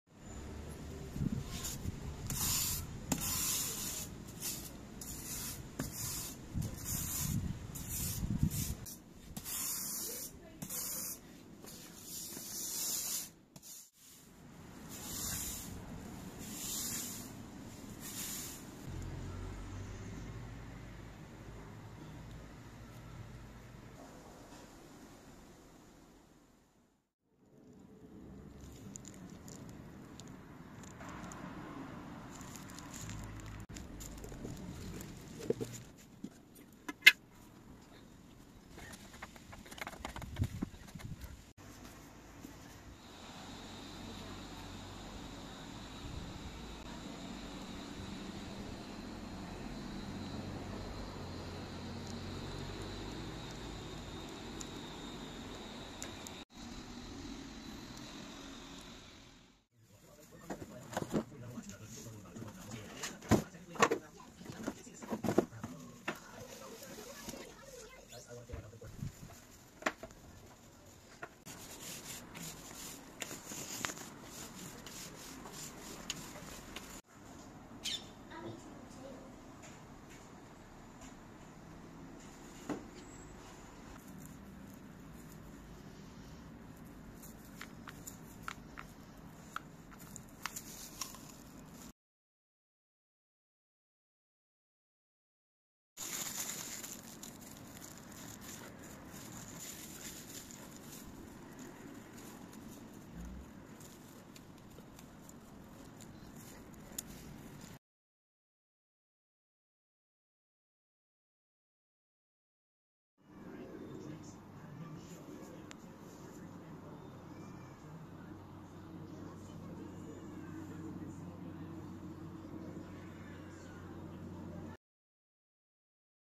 Garden makeover
outdoor patio paint (two coat on floor)
roller
brush cleaner
hard wire brush
stencil for design
tape and colour pencil for marking